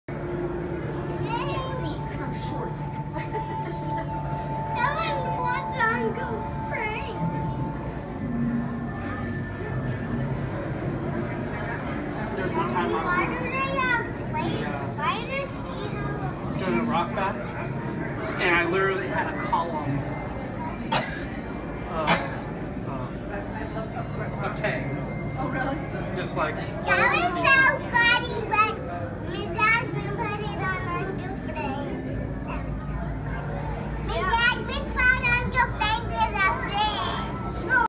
National Aquarium Atlantic Coral Reef Rock Bath
A few weeks ago I had the good fortune to get to dive the National Aquarium in Baltimore, MD.  I went diving in the Atlantic Coral Reef exhibit and some of the fish in there just looooooved getting a rock bath.  As you can see...